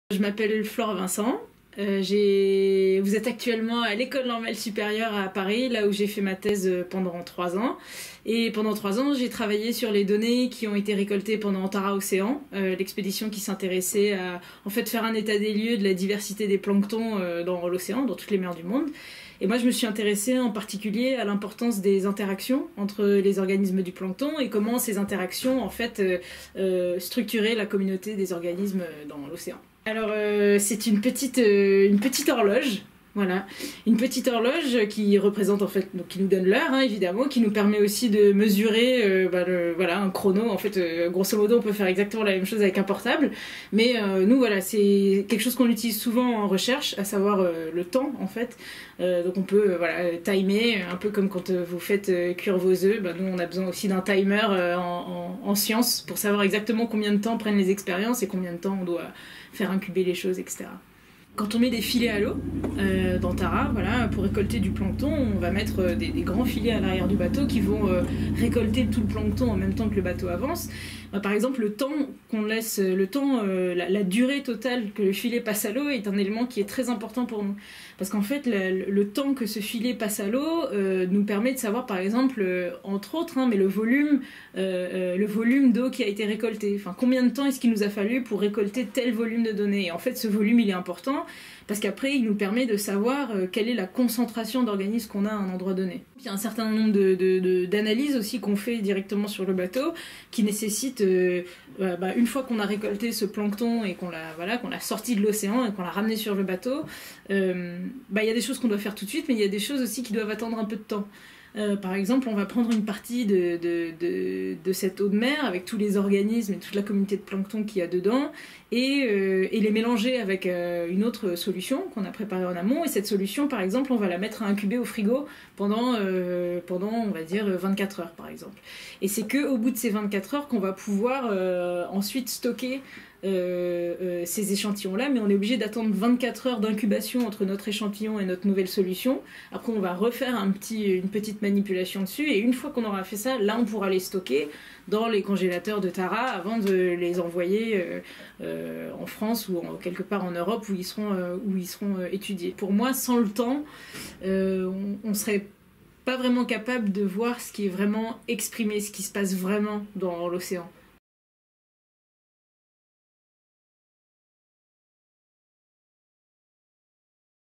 Coulisses de laboratoires - Horloge
"Interview de Flora Vincent, docteur en biologie marine qui nous parle de l'expédition Tara Pacific et de l'importance de la mesure du temps.
Réalisé dans le cadre de « Coulisses de laboratoire », une opération Tara qui vous plonge au cœur de l’investigation scientifique en plaçant au premier plan les instruments utilisés par les chercheurs. "